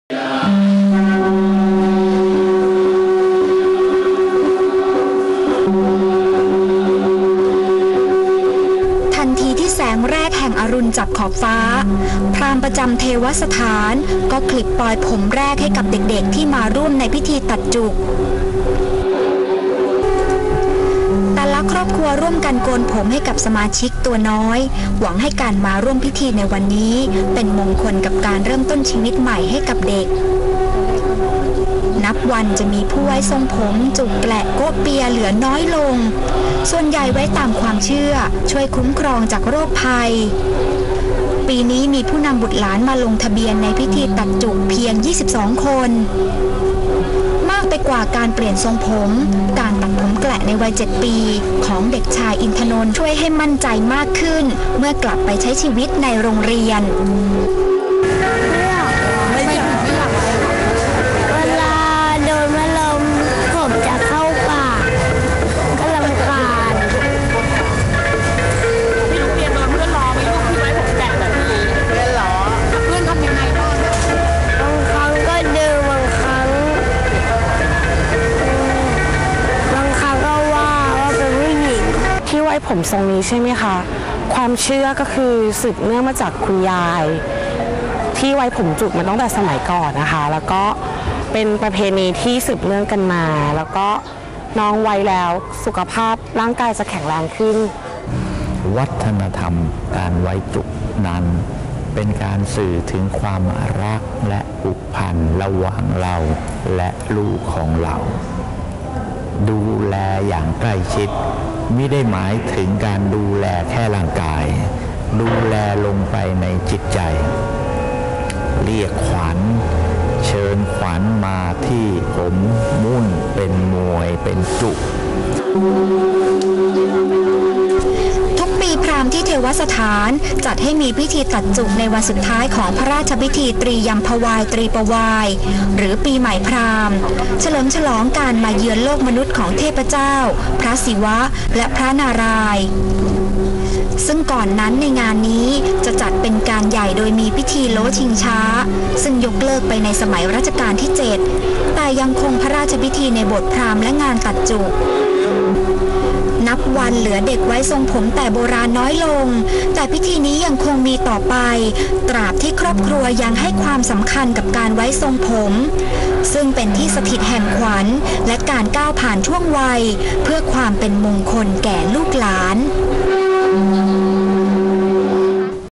พิธีตัดจุกที่โบสถ์พราหมณ์
ทุกวันนี้ทรงผมเด็กไทยแต่โบราณ เช่น จุก แกละ โก๊ะ มีให้เห็นน้อยลง แต่ทุกปีในวันสุดท้ายของงานพิธีตรียัมพวาย ตรีปวาย หรือ ปีใหม่พราหมณ์  จะได้เห็นเด็กกลุ่มหนึ่งซึ่งยังคงไว้ทรงผมแต่เดิมนี้ มาร่วมพิธีตัดจุกที่โบสถ์พราหมณ์ เป็นธรรมเนียมที่สืบทอดมายาวนาน และสะท้อนสายสัมพันธ์ในครอบครัวค่ะ

ออกอากาศทางข่าวค่ำ ThaiPBS ช่วงไทยบันเทิง 21 ม.ค. 57 19.40 น.